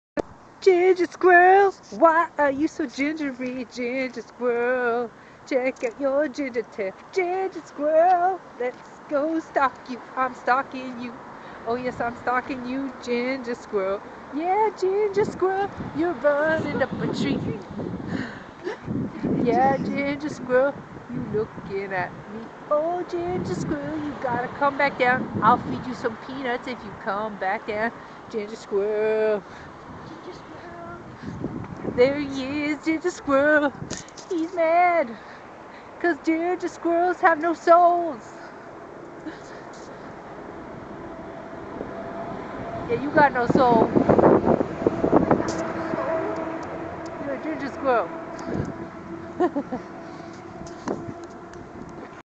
ginger squirrels have no souls
i met a ginger squirrel at the park who had no soul.